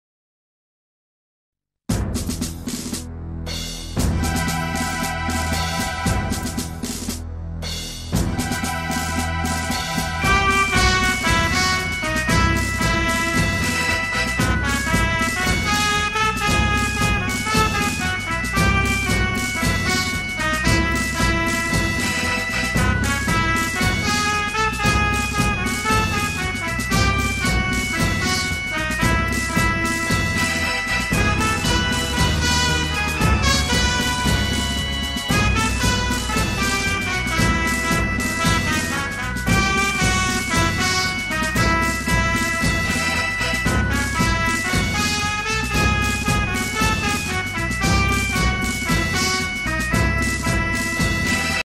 Happy independence day 2017: A message from central calcutta polytechnic
freedom in our mind ,faith in the word,pride in our souls,let's salute the great men and woman who made this possible.hai hind.